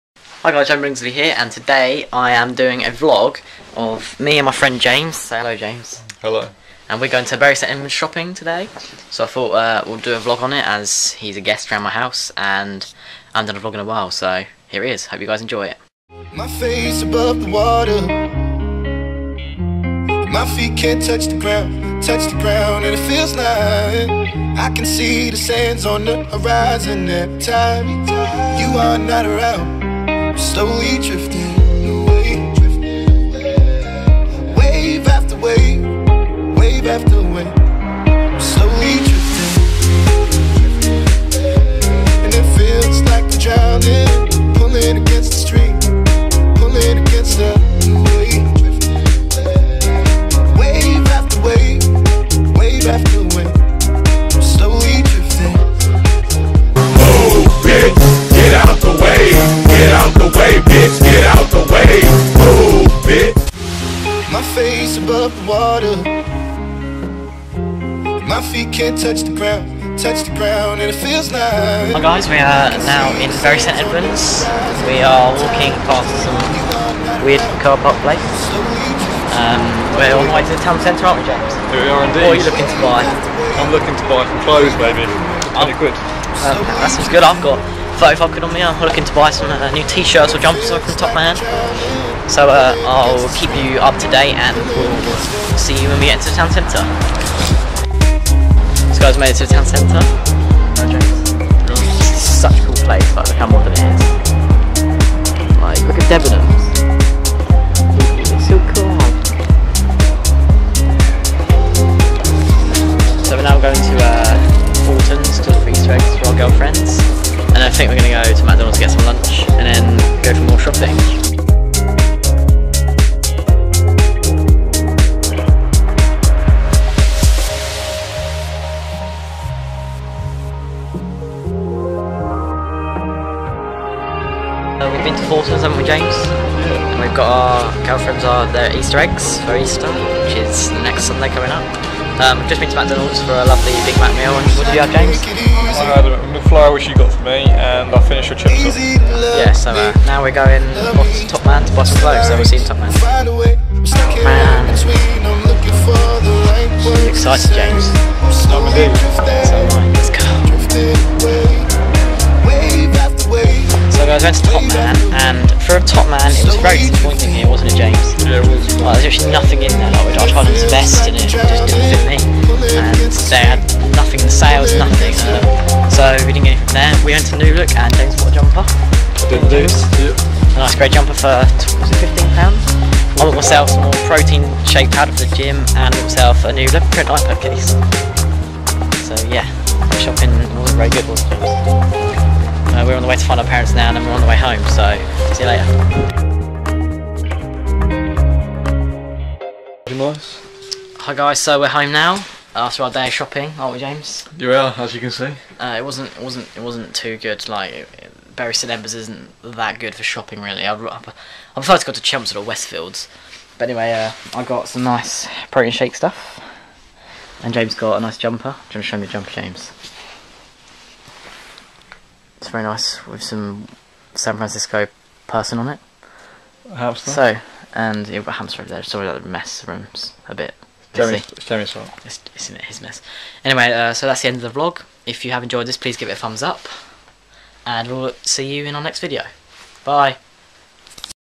A Trip to Bury St Edmunds With James
Went to Bury St Edmunds with James and decided to Vlog, So here it is!